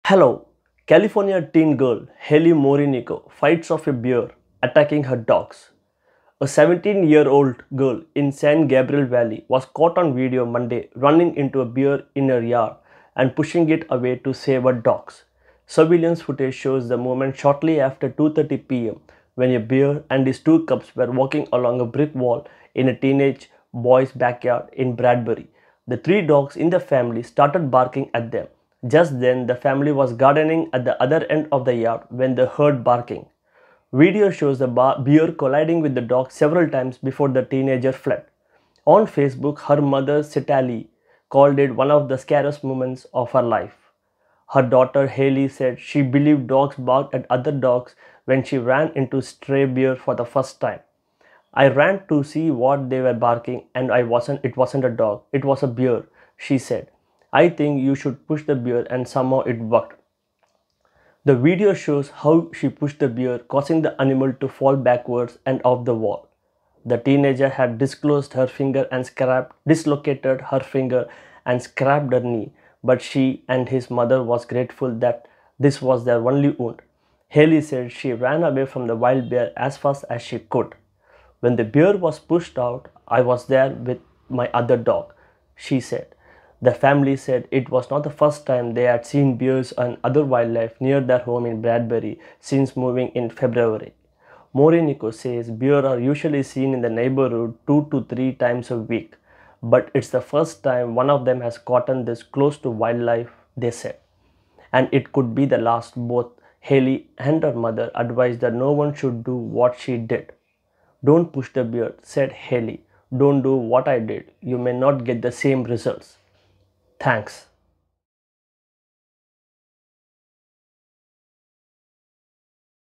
California teen girl Hailey Morinico fights off a bear attacking her dogs
2/6/21